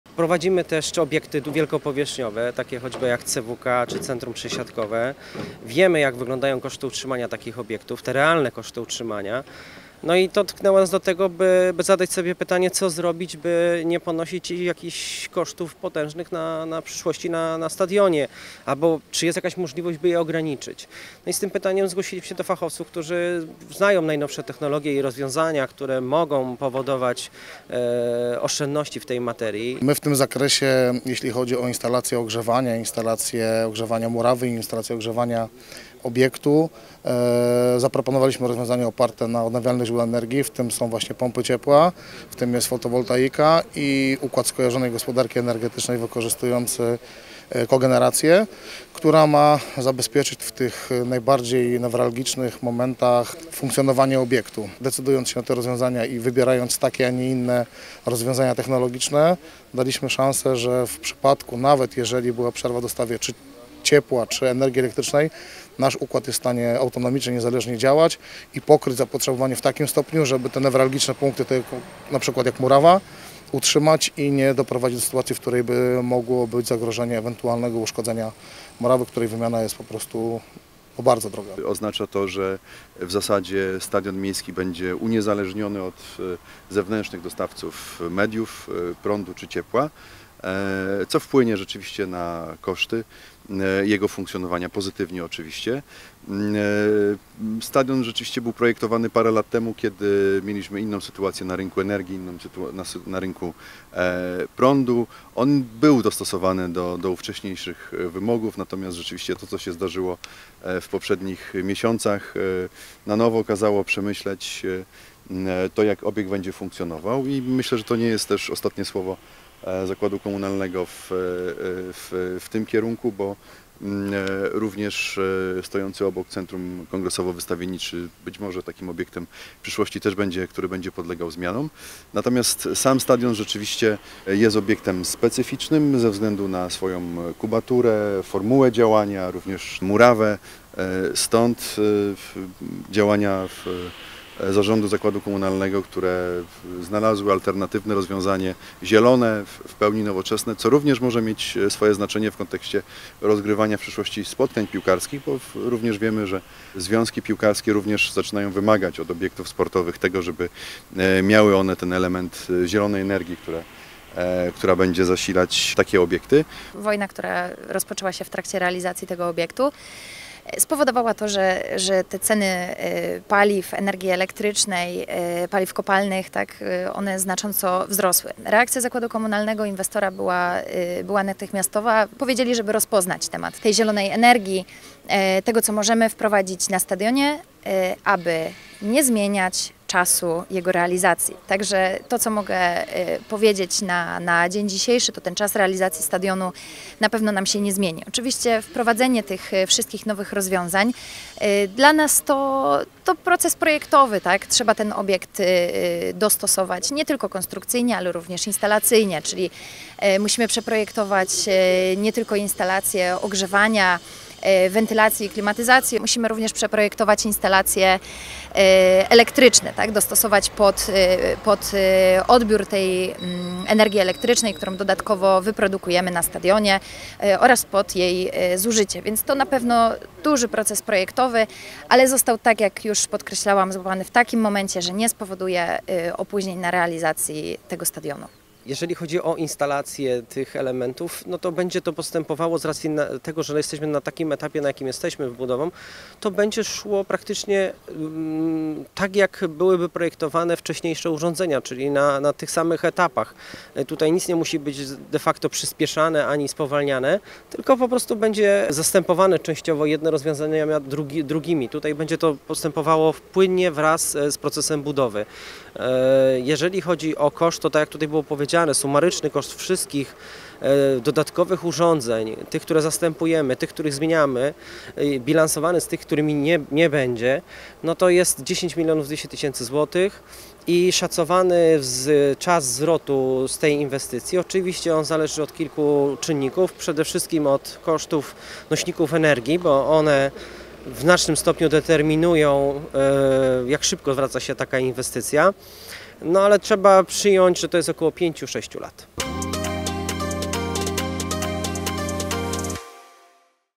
Stadion Opolski z energią odnawialną
W związku ze wzrostem cen spowodowanymi sytuacją na świecie, w projekcie budowy stadionu Opolskiego zostaną naniesione zmiany dotyczące eksploatacji tego obiektu, a konkretniej zmniejszenia jej kosztów.  Wprowadzony zostanie układ składający się z fotowoltaiki, pomp ciepła oraz agregatów z funkcją kogeneracji., To w dużej mierze uniezależni stadion od konwencjonalnych źródeł energii i pozwoli wykorzystywać w tym obiekcie źródła energii odnawialnej. Warto dodać, że projekt jest absolutnie nowatorski w skali kraju, dla tego typu obiektów sportowych i nie tylko.